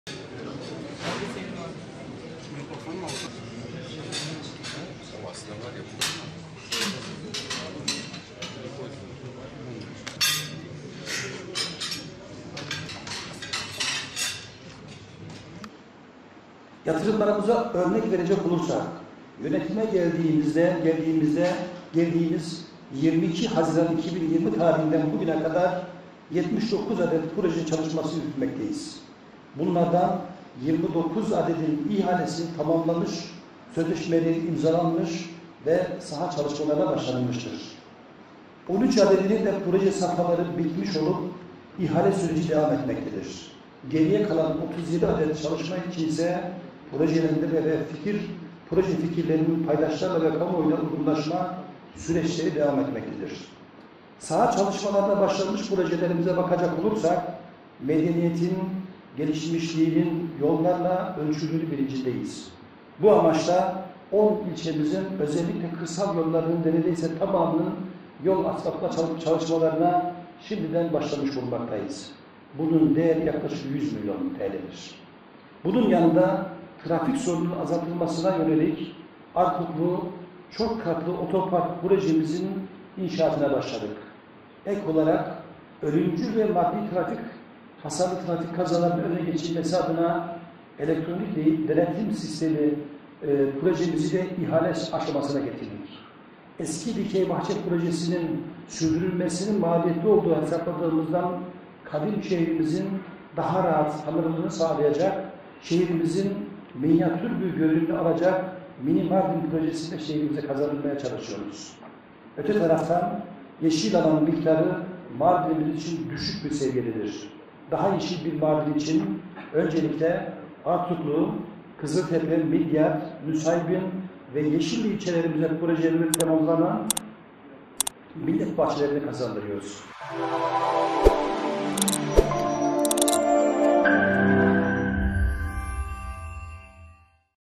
Mardin Valisi Demirtaş basın mensuplarına bir yıllık faaliyetlerini anlattı
Mardin Valisi ve Büyükşehir Belediye Başkan Vekili Mahmut Demirtaş, basın mensupları ile bir araya gelerek bir yıllık faaliyetlerini anlattı.

Değerli İzleyicilerimiz;